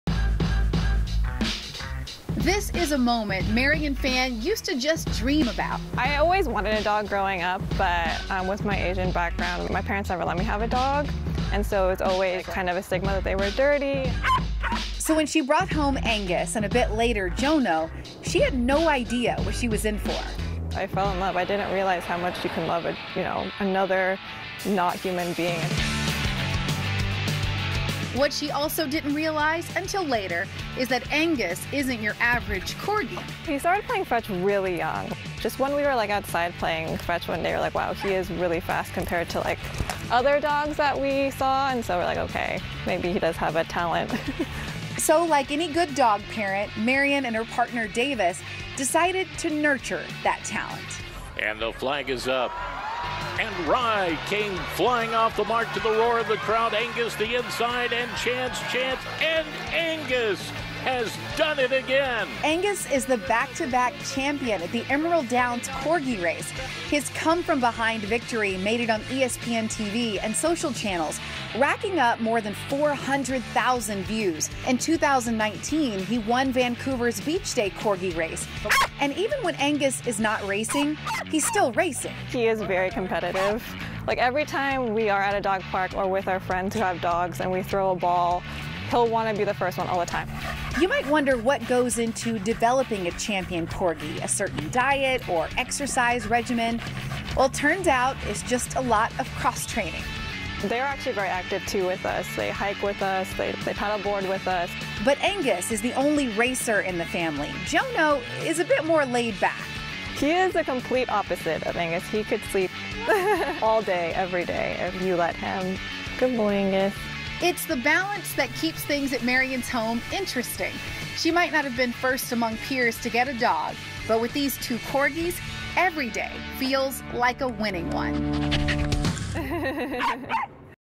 This adorable corgi is a back to back champion at Emerald Downs
Marian Phan and her partner Davis suspected their new dog Angus was a bit different than other dogs his age.

"He started playing fetch really young. He loved playing ball and frisbee and we were like 'Wow, he's fast compared to other dogs that we saw', so we thought maybe he does have a talent," explained Phan.

Turns out, they were right.

The Phan's entered him into the Emerald Downs annual Corgi Race. He did not win the first year because he got distracted.  But in 2019, he took home the championship. He did it again in 2021, beating out 70 other Corgis to take home the title.

He captured national attention, appearing on ESPN, WGN and on several other major TV networks.

And even when he's not racing, Angus still races.

"He's very competitive. Every time we are at a dog park or with friends who have dogs, he will want to be first and he will bark at other dogs, so he is a competitive dog," shared Phan.

It's fair to wonder what goes into developing a champion Corgi. Is it the diet or an exercise routine?

Turns out it's just a lot of cross-training.

"They are active.  They hike with us. They paddleboard with us," shared Phan.

Angus is now five years old and the Phans are pondering retirement.  He and his sibling, Jono have an Instagram account for those interested in following their journey.